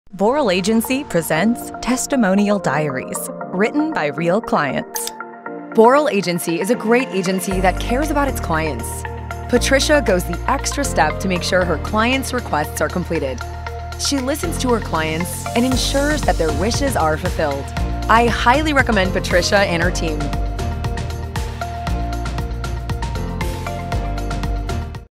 Boral Agency Presents Testimonial Diaries: Cynthia F
Real reviews of Boral Agency written by real clients! In this video, we learn about a happy client and her experience working with Boral Agency.

Here at Boral Agency, we Rethink Brands. Create Strategies. Bring Results.